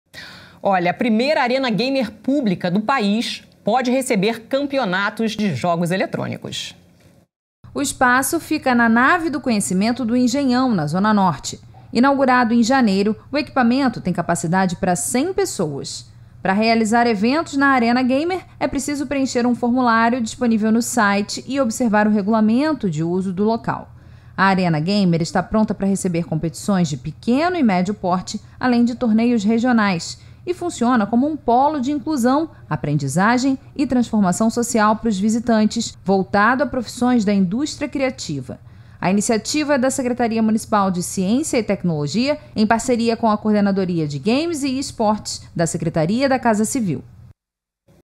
Nave do Conhecimento recebe primeira Arena Gamer do país | Câmara Rio Notícias
Câmara Rio Notícias (05/03/2024): 🎮A primeira Arena Gamer pública do país pode receber campeonatos de jogos eletrônicos.

O espaço fica na Nave do Conhecimento do Engenhão, na zona norte. Inaugurado em janeiro, o equipamento tem capacidade para 100 pessoas. Para realizar eventos na Arena Gamer, é preciso preencher um formulário disponível no site e observar o regulamento de uso do local. A Arena Gamer está pronta para receber competições de pequeno e médio porte, além de torneios regionais. E funciona como um  polo de inclusão, aprendizagem e transformação social para os visitantes, voltado às profissões da indústria criativa. A iniciativa é da Secretaria Municipal de Ciência e Tecnologia, em parceria com a coordenadoria de games e e-sports, da secretaria da Casa Civil.